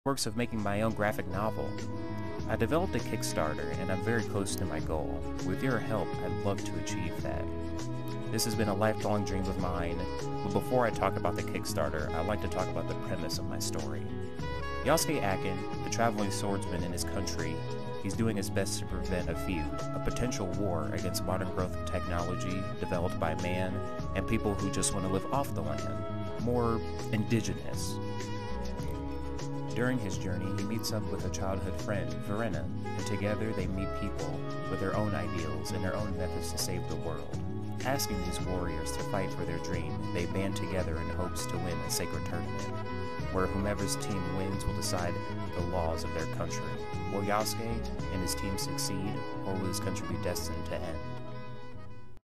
Kickstarter Campaign Toura Warriors Rebirth
This something a bit different than my spider-man content. Trying to make a graphic novel, a comic book and developed a Kickstarter to help fund for my story. This would mean a lot if you could take the time to look into my kickstarter. Here's my link